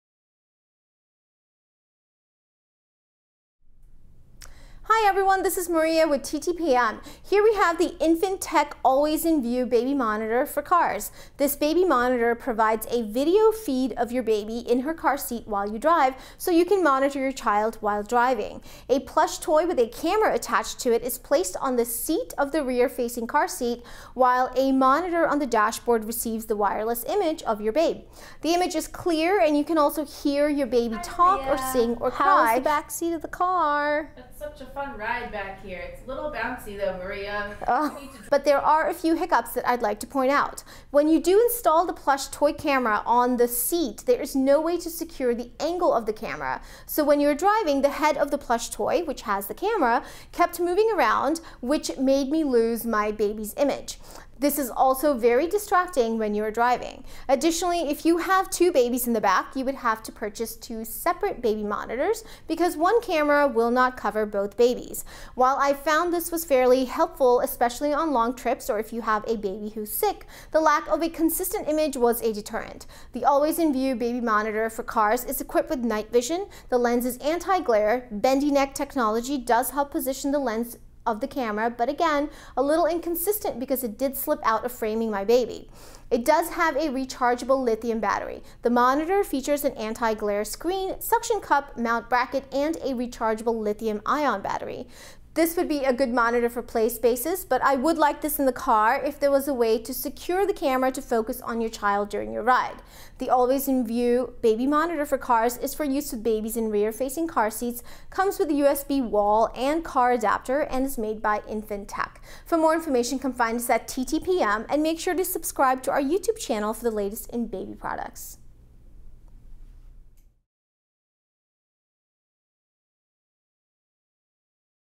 Always In View Baby Monitor for Cars from Infanttech
The Always in View Baby Monitor for Cars provides a video feed of your baby in her car seat, so you can monitor your child while you drive. For full review and shopping info

Product Info:
The Always in View Baby Monitor for Cars provides a video feed of your baby in her car seat, so you can monitor your child while you drive. A plush toy with a camera attached to it is placed on the seat of the rear-facing car seat. A monitor that suction cups onto the dashboard receives the wireless images of your babe. The image is clear, and you can also hear your baby talk, sing, or cry. The Always in View Baby Monitor is equipped with night vision and an anti-glare lens. It runs on a rechargeable lithium battery and comes with two USB cables, a USB car charger, and an AC adapter. It's available in three styles: puppy, giraffe, and monkey. Each is sold separately.